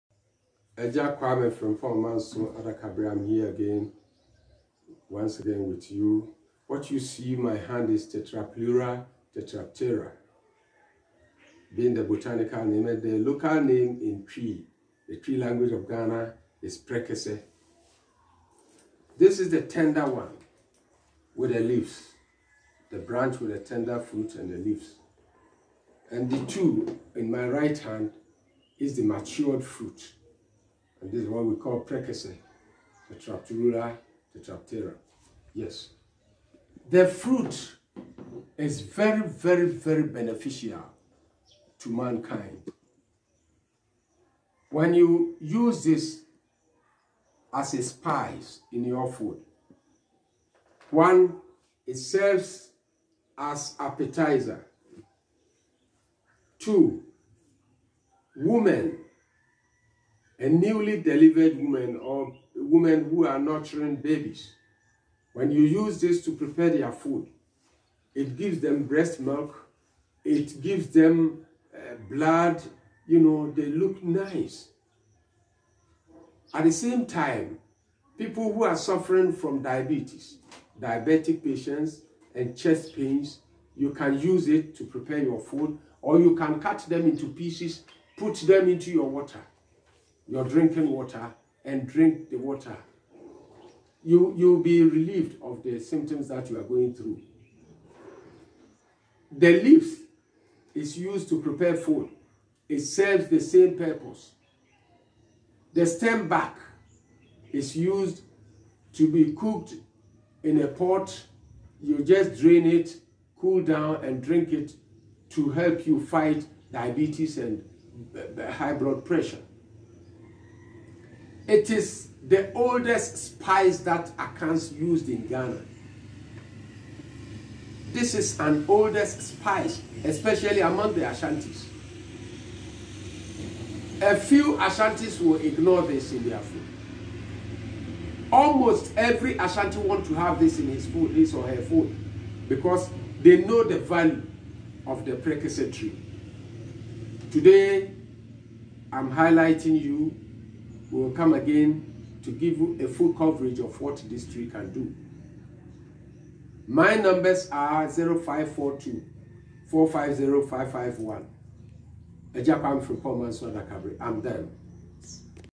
July 5, 2021
well used appetizer from the tetrapleura tetraptera tree which has been in use on the western part of Africa for hundreds of years.      it has the potency of healing many ailments. give it a try and be a witness.